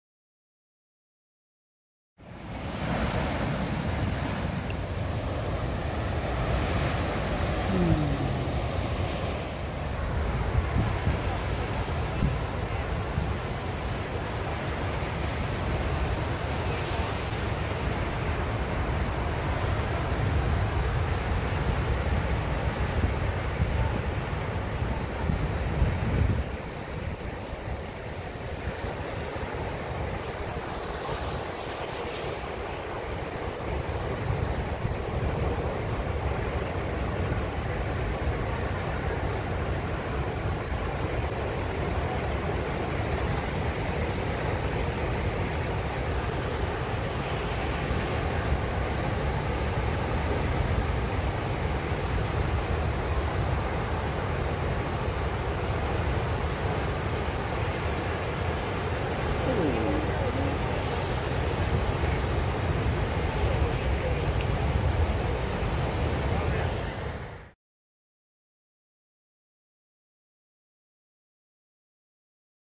Banzai Pipeline surf break on a good day - short video
Clips of the beach and surf at Pipeline on Oah'u in Hawaii